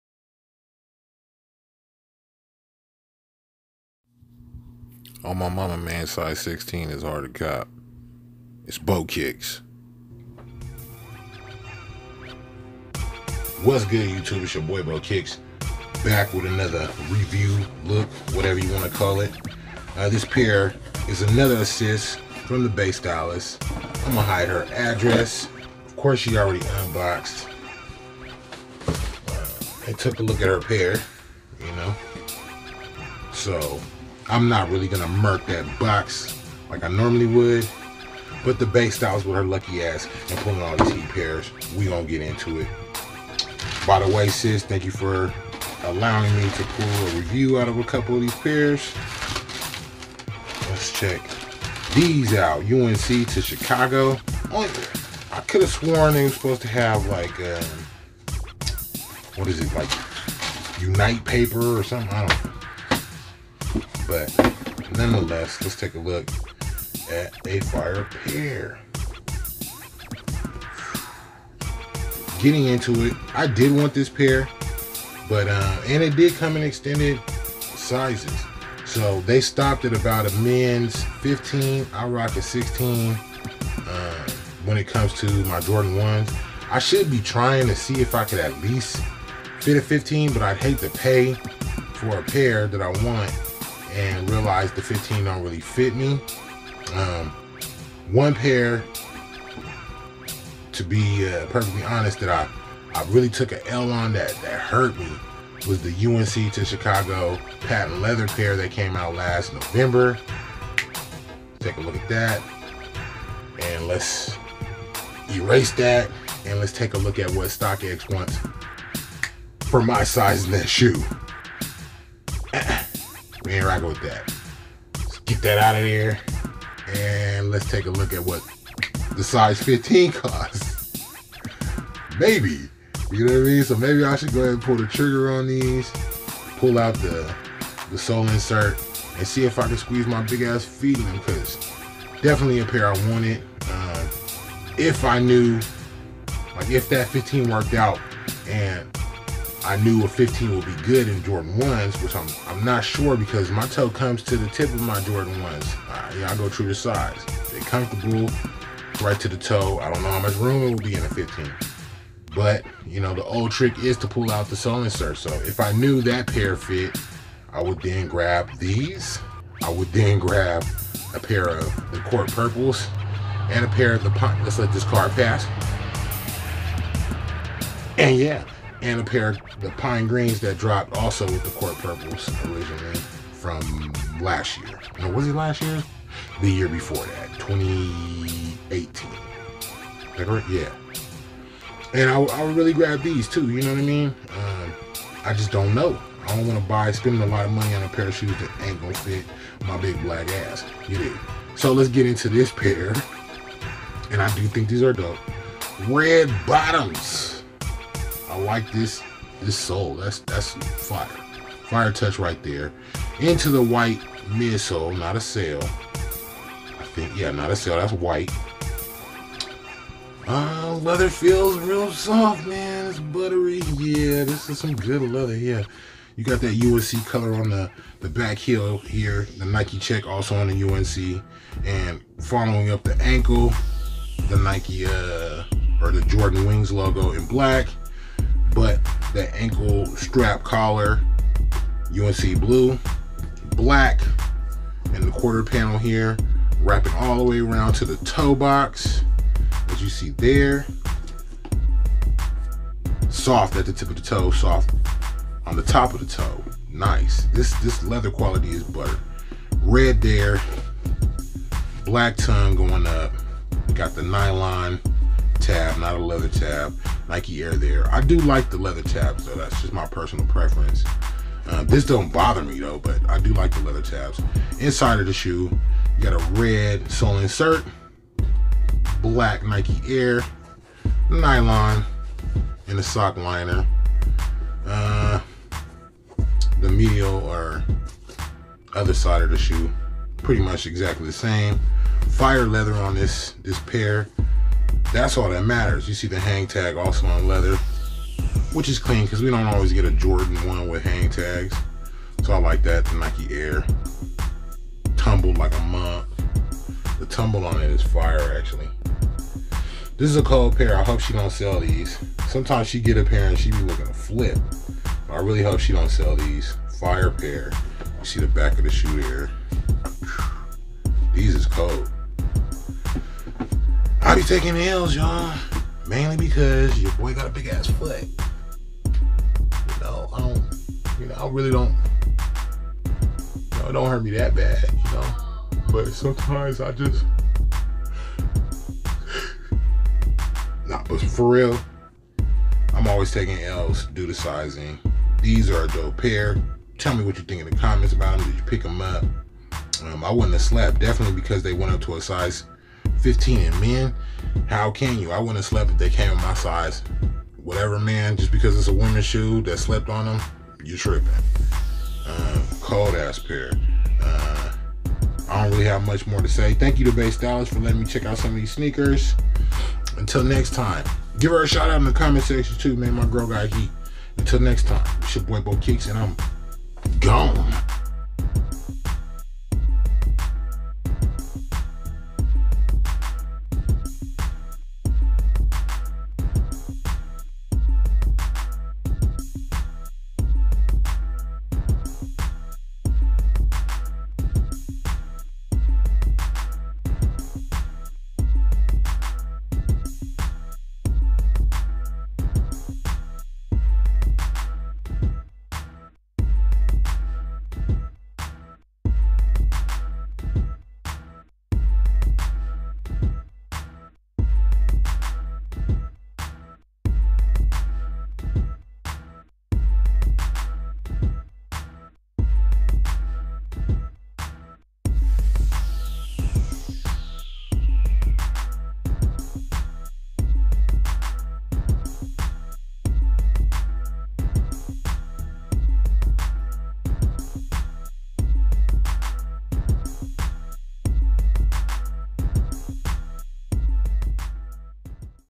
JORDAN 1 UNC TO CHI LOOK AND REVIEW
The 3rd Assist From My Folks The Bay Stylist. What Did I Think Of This Extended Size Womens Pair ?